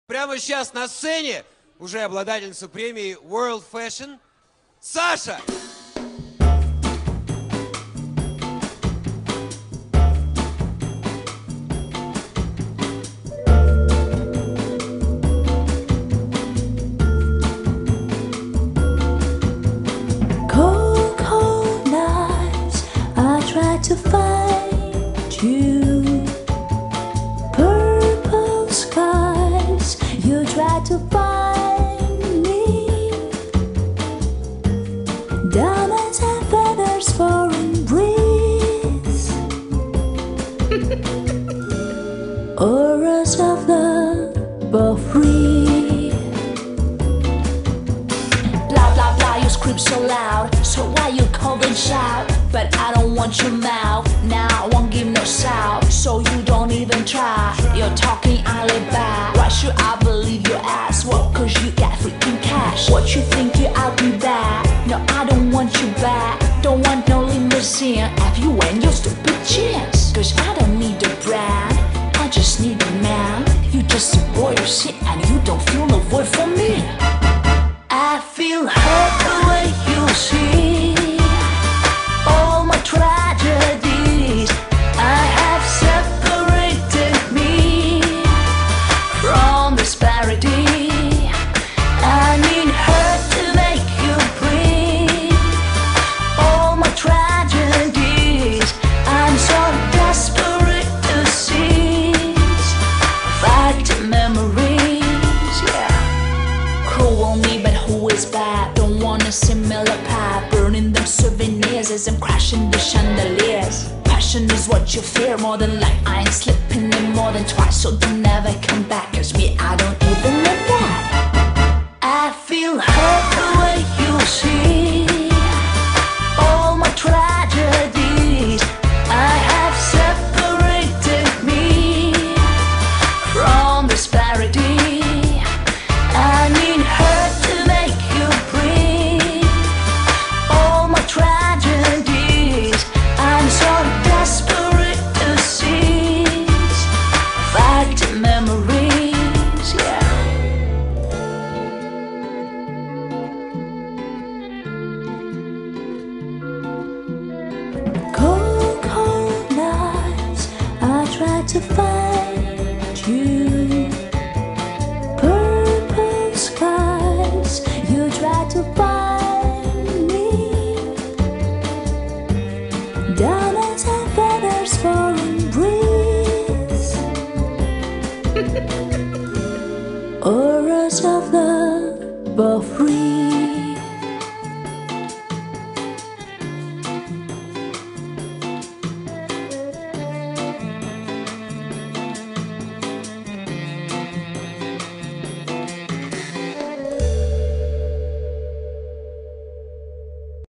Sasha singer performs on World Fashion Awards ceremony
Выступление певицы Саши на церемонии World Fashion Awards